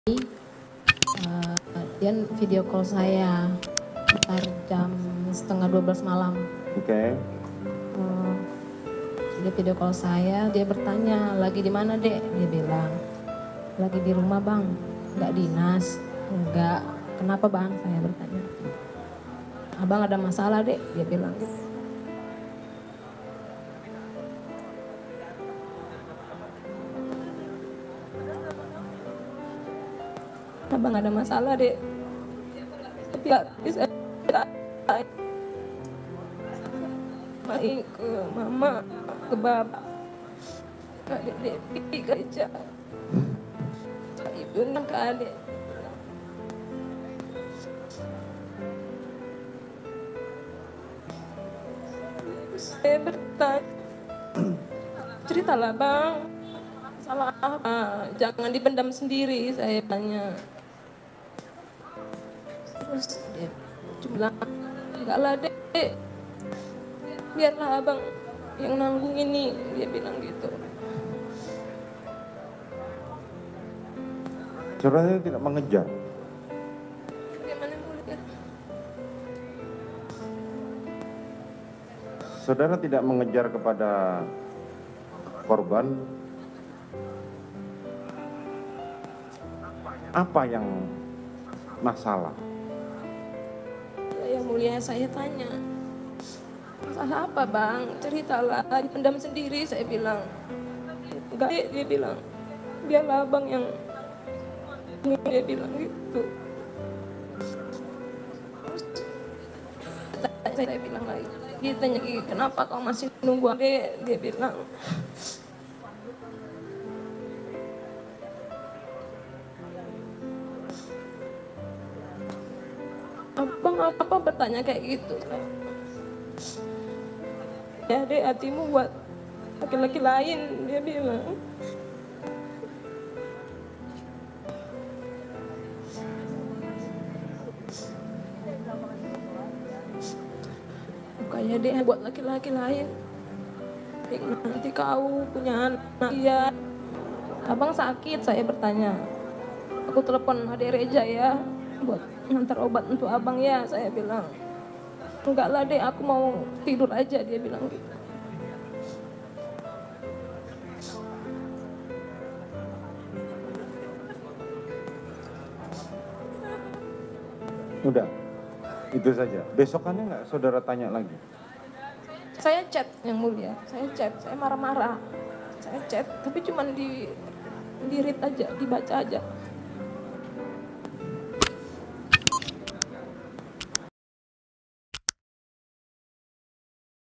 Curhatan Brigadir J ke Kekasih Vera Simanjuntak
SERAMBINEWS.COM - Dalam sidang kedua Bharada E yang digelar Selasa (26/10), kekasih mendiang Brigadir Yosua Hutabarat, Vera Simanjuntak memberikan kesaksian.

Sempat disebutkan bahwa dirinya sering mendengar cerita dari Brigadir J, dan seringkali mendapati Brigadir J dengan sikap yang menunjukkan dirinya memendam masalah.

Selain itu, Kuasa Hukum Bharada Eliezer, Ronny Talapessy, memberikan tanggapan setelah Pengacara Brigadir Yosua, Kamaruddin Simanjuntak bersaksi di persidangan hari ini, Selasa (25/10).